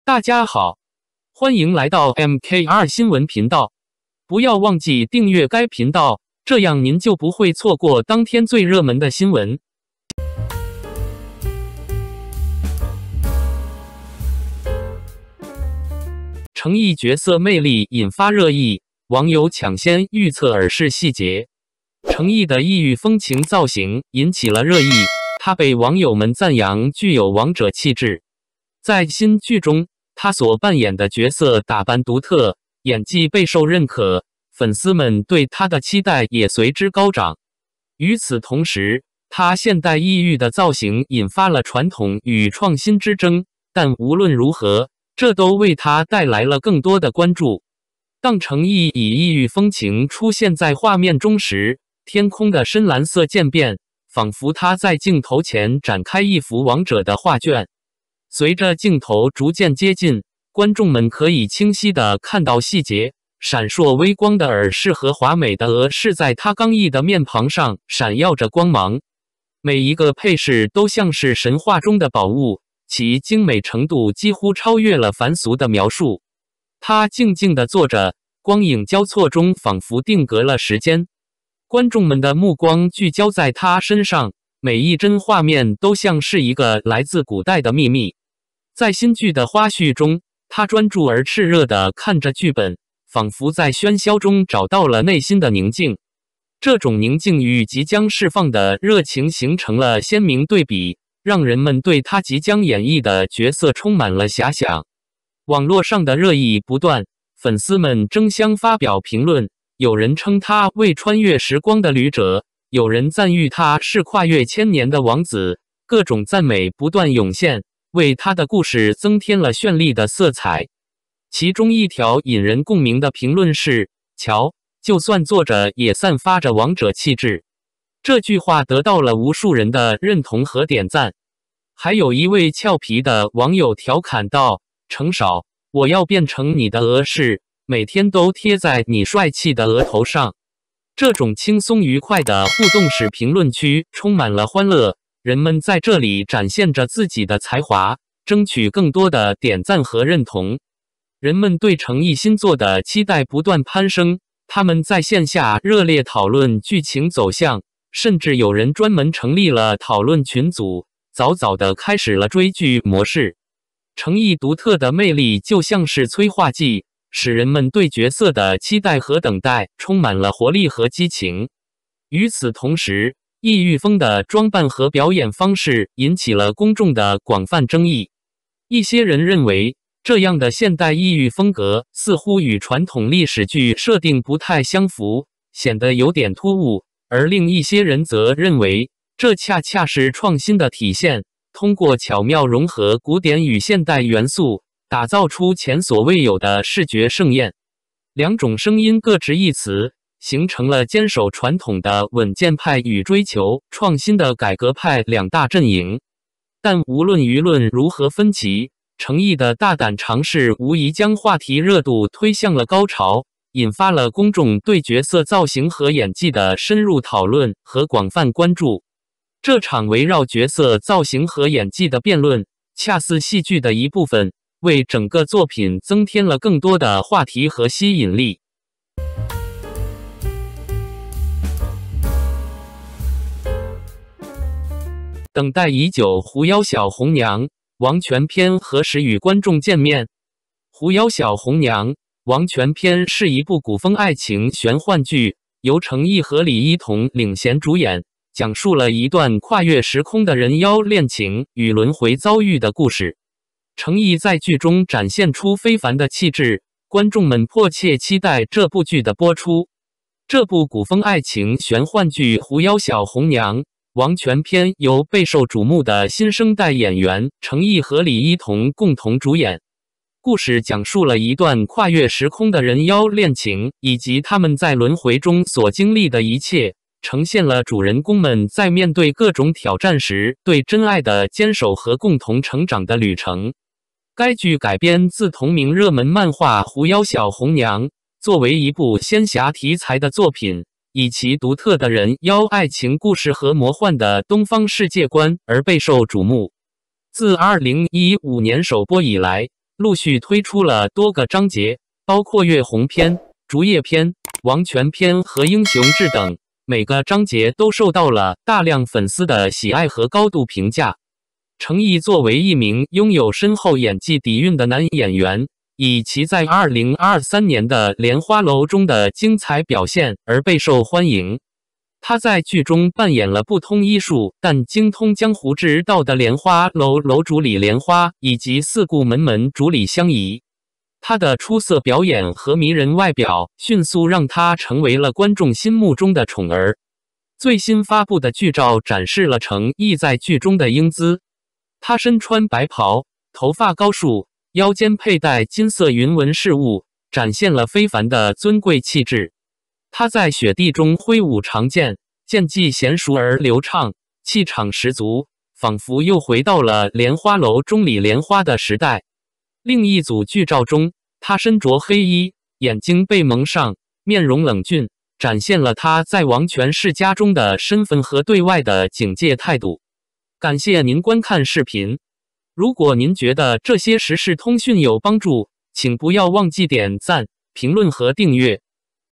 成毅 角色魅力引发热议，网友抢先预测耳饰细节！等待已久，《狐妖小红娘·王权篇》何时与观众见面？chengyi
成毅异域风采炸裂！网友疾呼：剧情未卜耳饰已被我锁定！
《狐妖小红娘·王权篇》离我们还有多久，你是否期待这部剧？